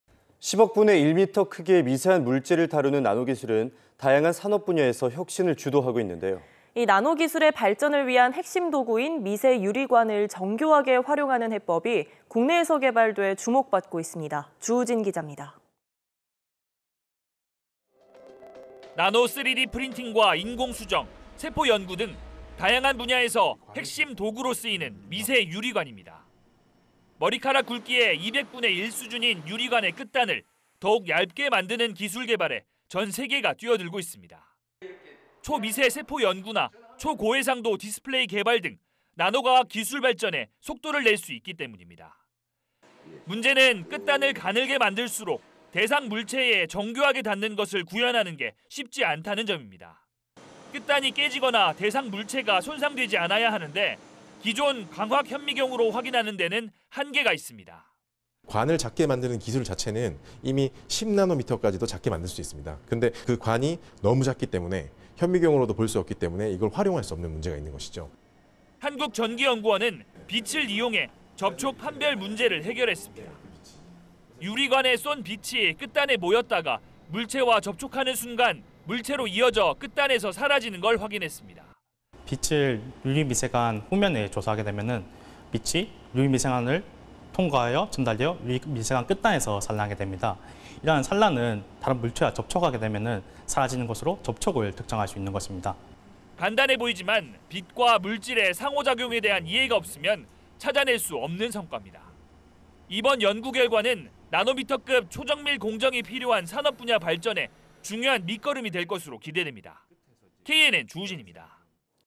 나노과학기술 숙제 해결, 혁신 기대/ KNN
10억 분의 1미터 크기의 미세한 물질을 다루는 나노기술은 다양한 산업 분야에서 혁신을 주도하고 있습니다.

제보
카톡 : 'KNN' 친구추가

검색 : KNN